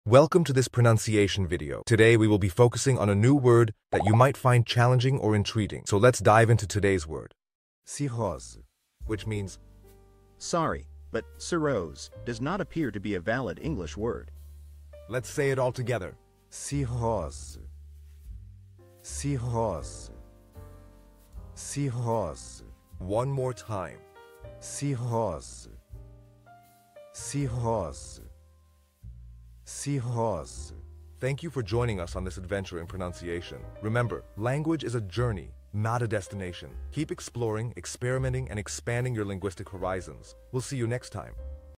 How to pronounce Cirrose
Master the Pronunciation of 'Cirrose WHICH MEANS CIRROSIS' - which means : Sorry, but "cirrose" does not appear to be a valid English word. 📝 with @PortuguesePronunciation 🎓🗣️ - [Your Guide to Portuguese]

Learn to pronounce 'Cirrose' perfectly in Portuguese with our easy and engaging tutorial by @PortuguesePronunciation! 📖📽️ In this comprehensive video, we provide you with native speaker examples 🗣️👂 and phonetic breakdowns 🎵 to ensure you get the pronunciation just right.

How to say Cirrhosis in Portuguese